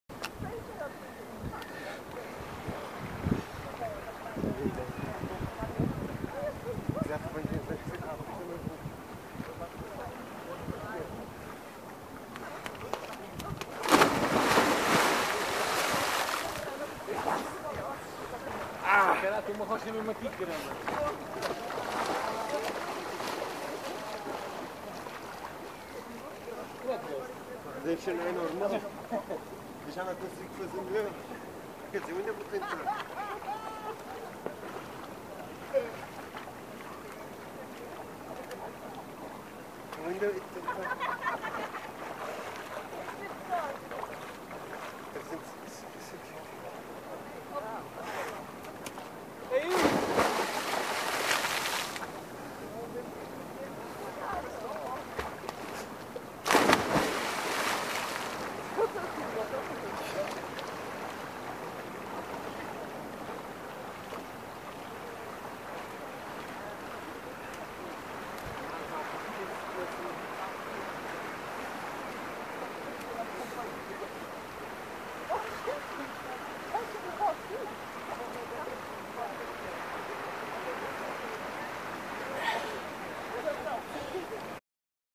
Xanaia na Fajã Grande - Flores
Filipe Soares e Nuno Machado numa sequência de Xanaias, na zona balnear da Fajã Grande, na ilha das Flores.